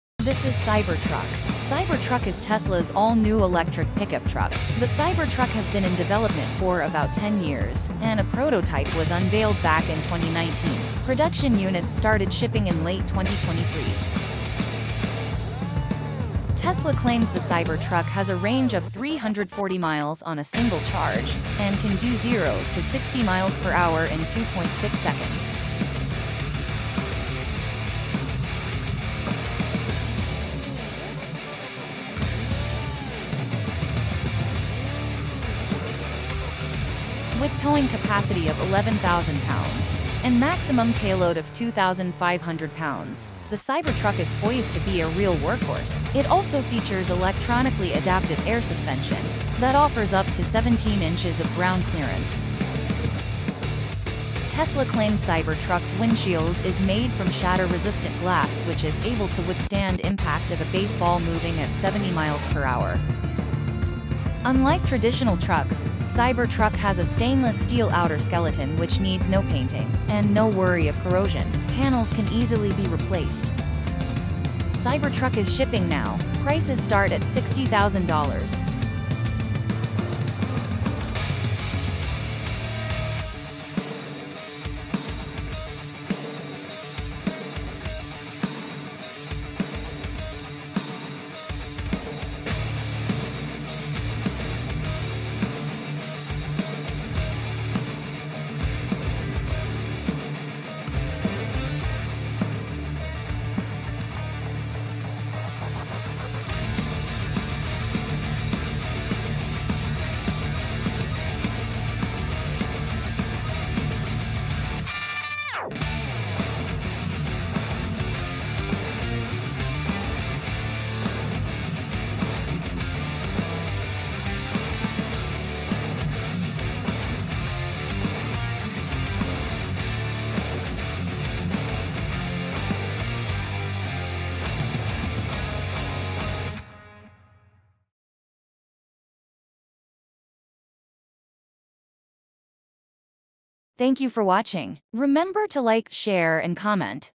Tesla Cyber Truck; what you need to know
Cyber Truck is tesla’s all new electric pickup truck.

The cyber truck has been in development for about 10 years and a prototype was unveiled back in 2019. Production units started shipping in late 2023.

Tesla claims the cyber truck has a range of 340 miles on a single charge and can do zero to sixty miles per hour in 2.6 seconds.

With towing capacity of 11,000 pounds and maximum payload of 2,500 pounds, the cyber truck is poised to be a real workhorse. It also features electronically adaptive air suspension that offers up to 17 inches of ground clearance.

Tesla claims cyber trucks windshield is made from shatter resistant glass which can withstand impact of a baseball moving at 70 miles per hour.

Unlike traditional trucks, cyber truck has a stainless steel outer skeleton which needs no painting and no worry of corrosion. panels can easily be replaced.

 Cyber Truck is shipping now. Prices start at $60,000.